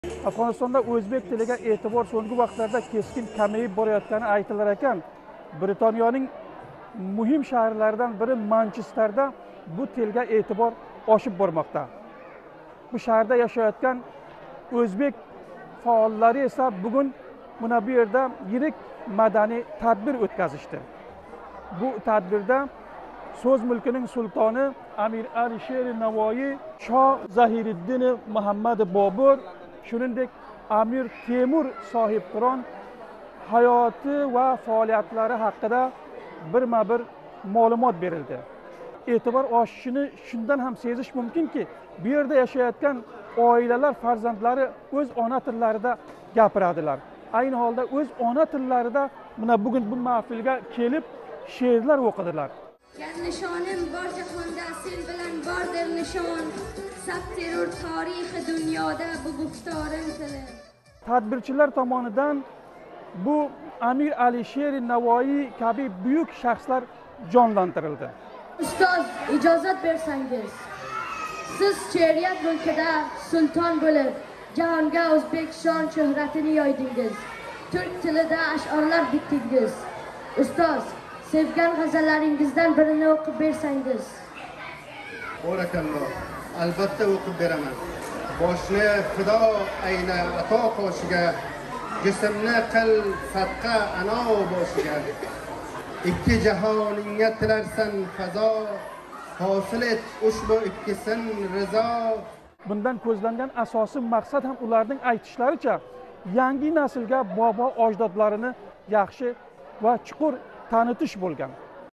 بریتانیاده اۉزبېک و تورکمنلرنینگ ییریک مدنی محفلی
اوشبو تدبیر خلق ارا آنه تیلی کونی همده سۉز ملکی نینگ سلطانی میر علی شیر نوایی، ظهیر الدین محمد بابر و امیر تېمور تولد ایامی مناسبتی بیلن اۉتکزیلدی.
بو کبی موضوعلرنی بی بی سی اۉزبېک تلویزیون کۉرستوویده افغانستان وقتی بیلن آقشام ساعت۶ ده تماشا قیلینگ.